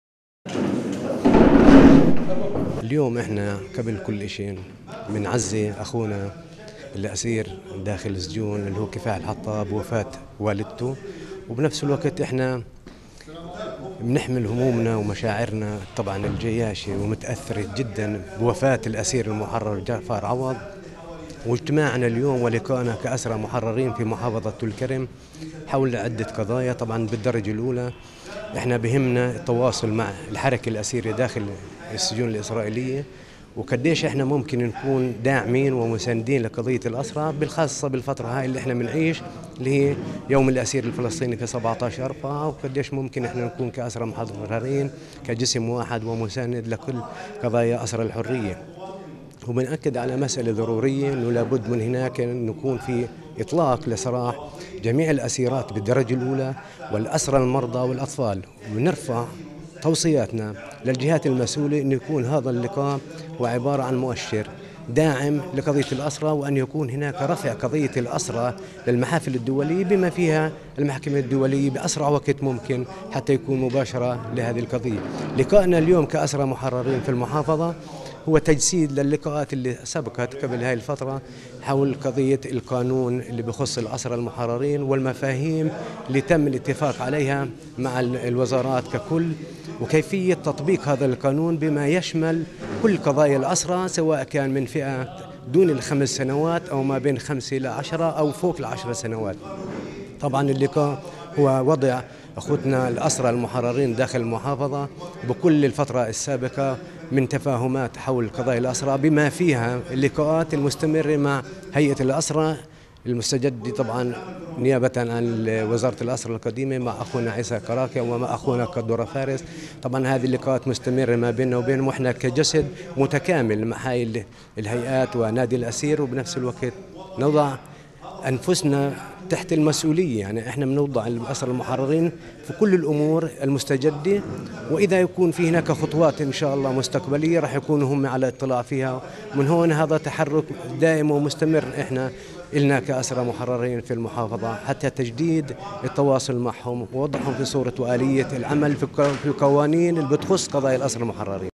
الاسرى المحررين يبحثون أوضاعهم خلال لقاء ضمهم اليوم في طولكرم
تلفزيون الفجر الجديد 11/4/2015 | الاسرى المحررين يبحثون أوضاعهم خلال لقاء ضمهم اليوم في طولكرم